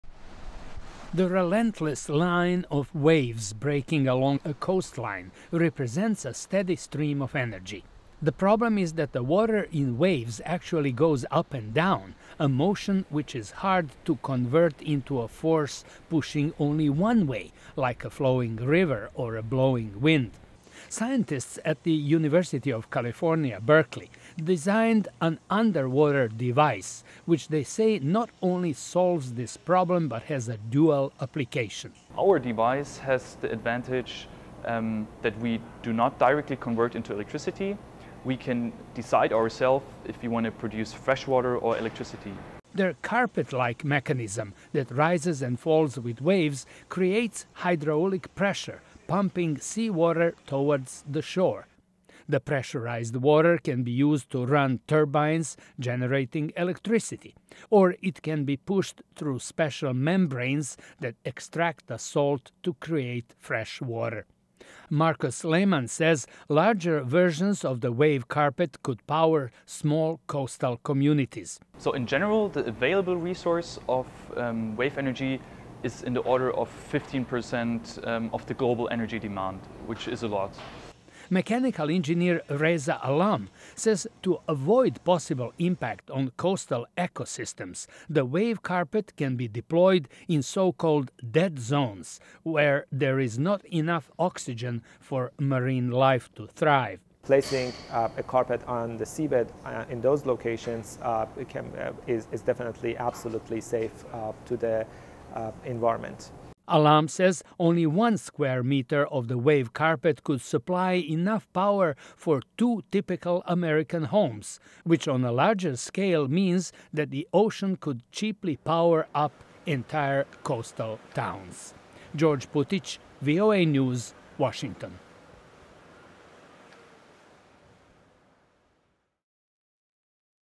Harnessing the Power of Ocean Waves
An often overlooked renewable energy resource is the power of ocean waves.  Scientists in California say harnessing energy from the regular movement of large amounts of water could provide cheap electricity and drinking water for coastal communities. VOA's George Putic reports.